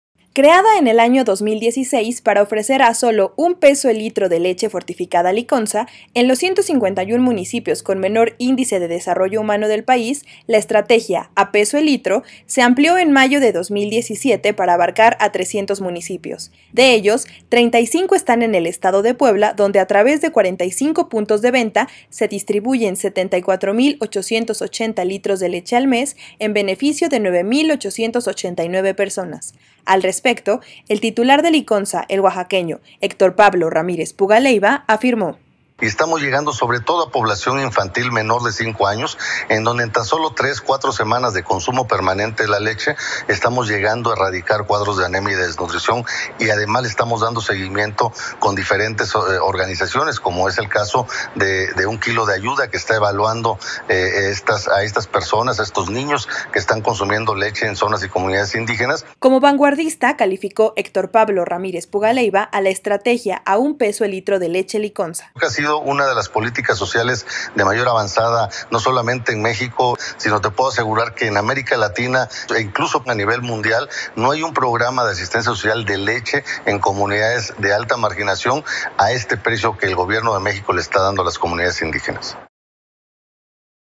La estrategia “A peso el litro” de leche fortificada Liconsa se amplió en mayo de 2017
Para abarcar a 300 municipios. De ellos, 35 están en el estado de Puebla, donde, a través de 45 puntos de venta, se distribuyen 74,880 litros de leche al mes, en beneficio de 9,889 personas.